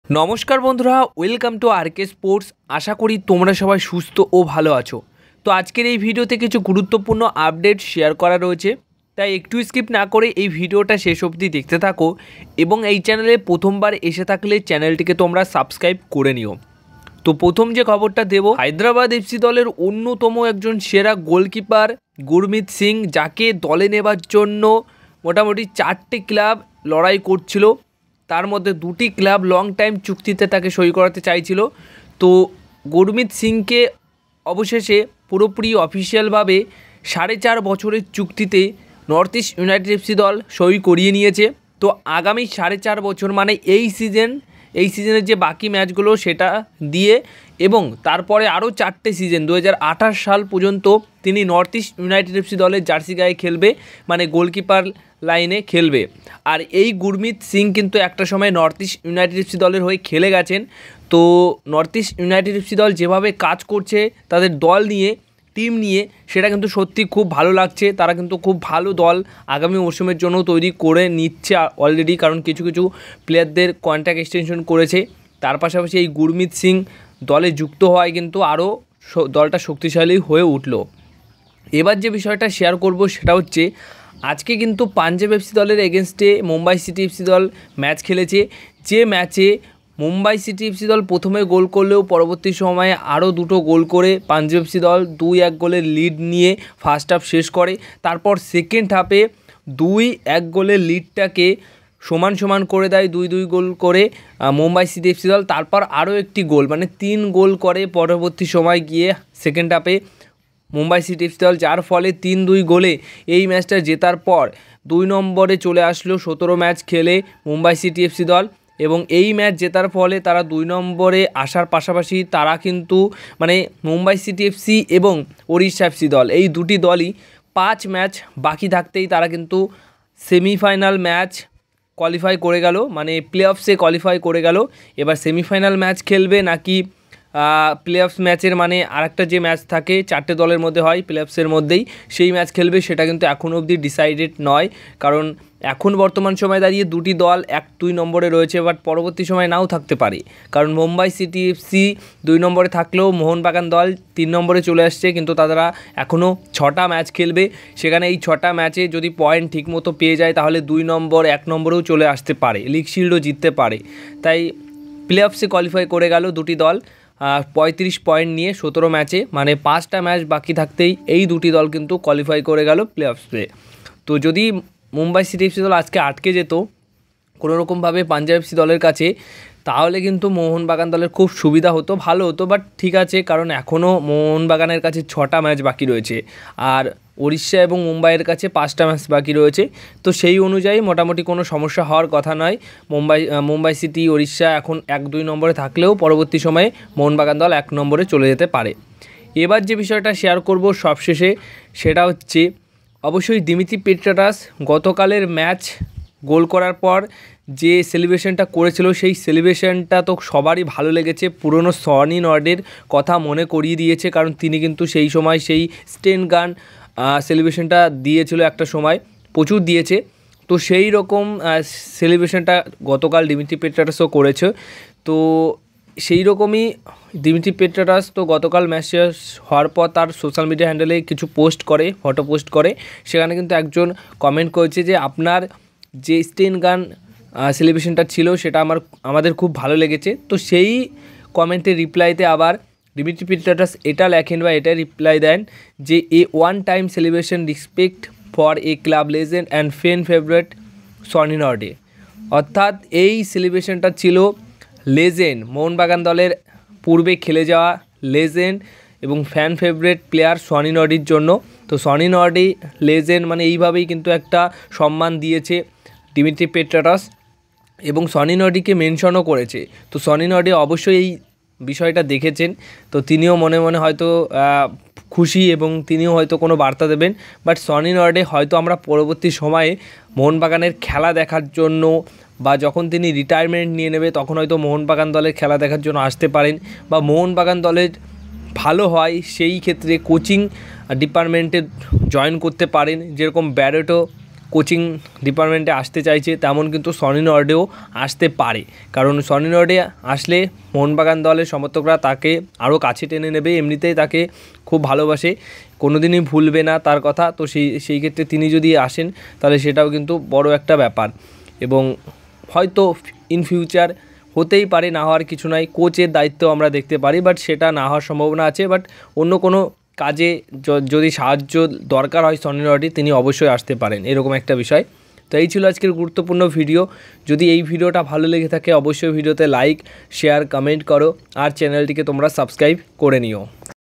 🚨 সনি কে নিয়ে বড়ো বার্তা Dimitri ❤️ Sony আবার ফিরবে| OFC ও MCFC কোয়ালিফাই করলো| MBSG News 🔥
🔵🔽photo collect(Image) :- ISL, MOHUN BAGAN FC ,Mohun Bagan, GOOGLE